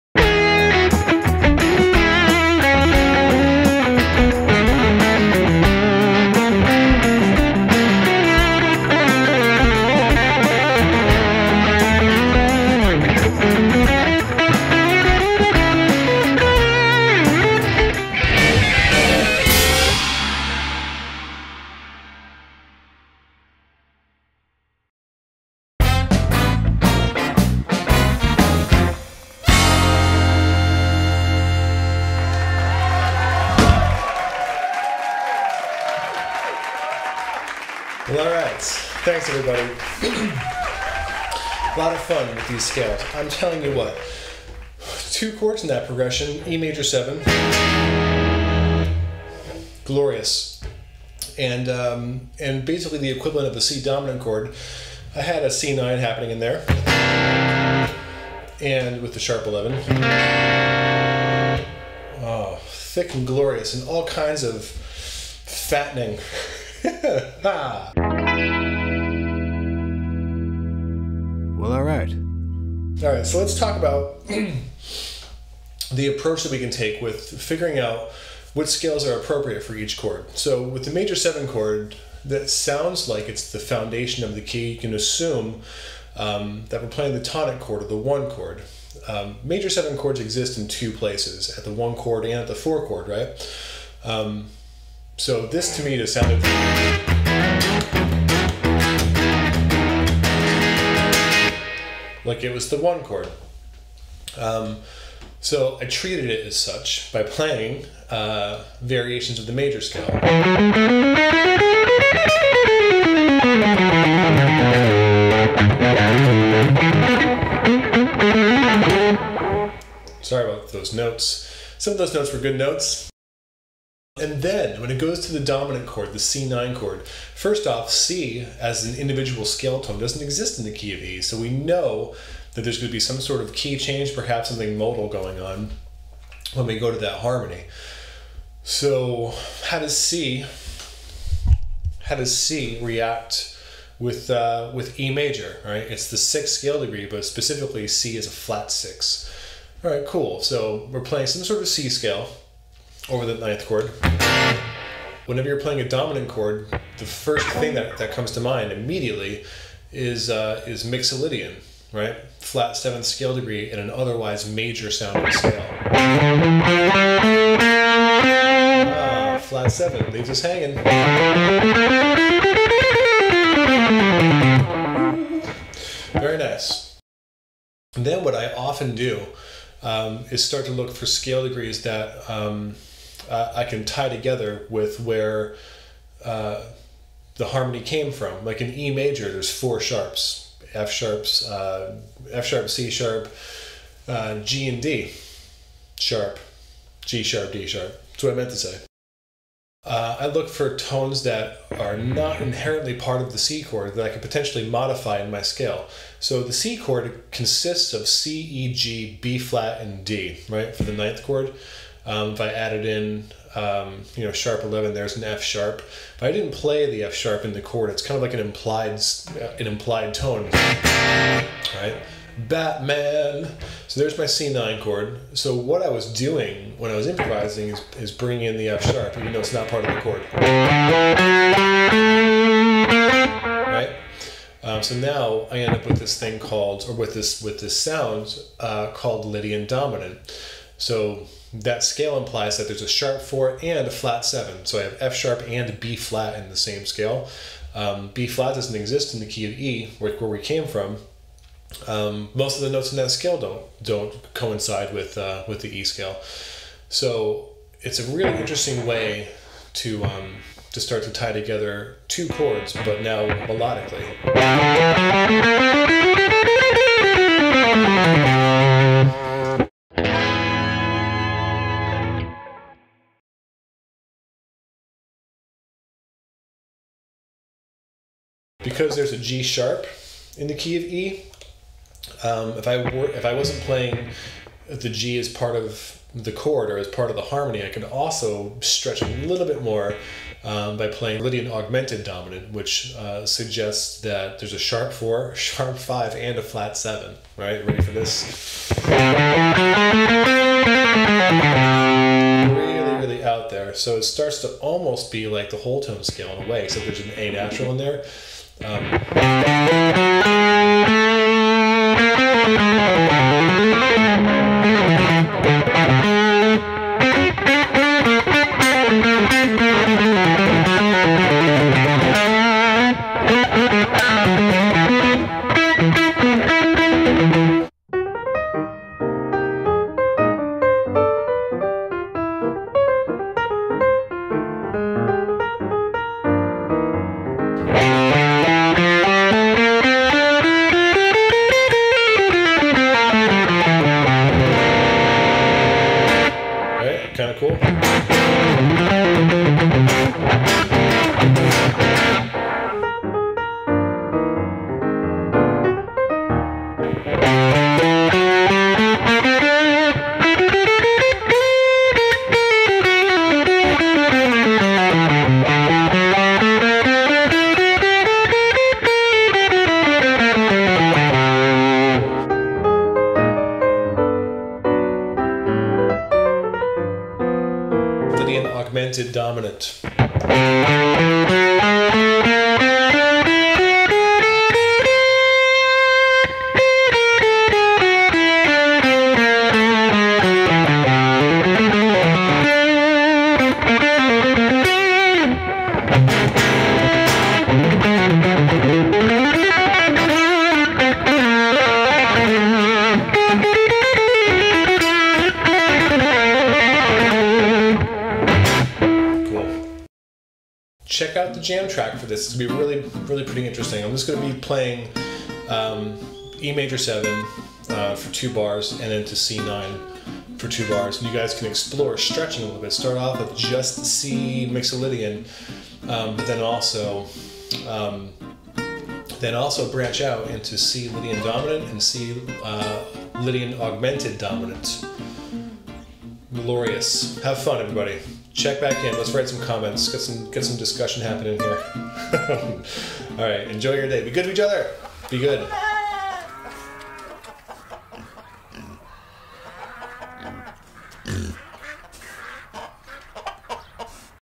Modal Improvising - Lesson One
In this glorious guitar lesson we begin to discuss which modes are appropriate for improvising in a non-diatonic setting. In other words, when you want to solo on your guitars over a progression of chords from totally unrelated keys, how do you solve that dilemma and play notes that work? Ahhh, very good! Get to work..